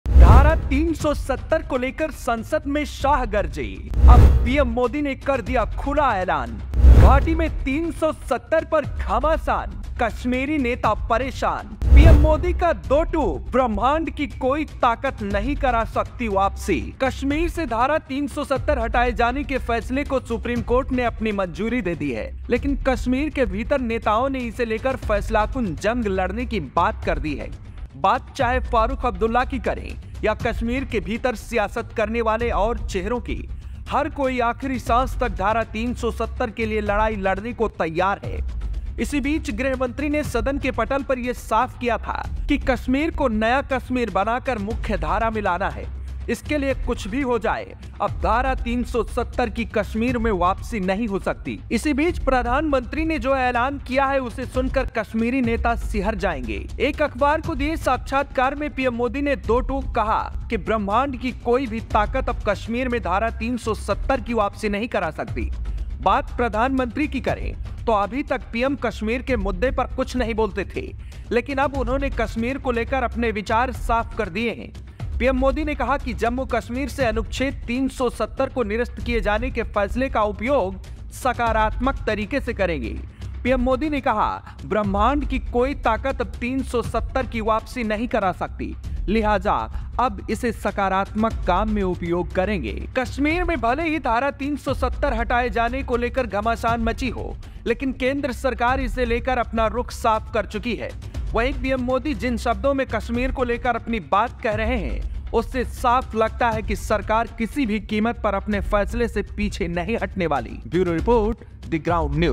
धारा 370 को लेकर Pm का दो टूक, ब्रम्हांड की कोई ताकत नही करा सकती वापसी | Article 370 | Pm Modi
"*"Copyright Policy"*"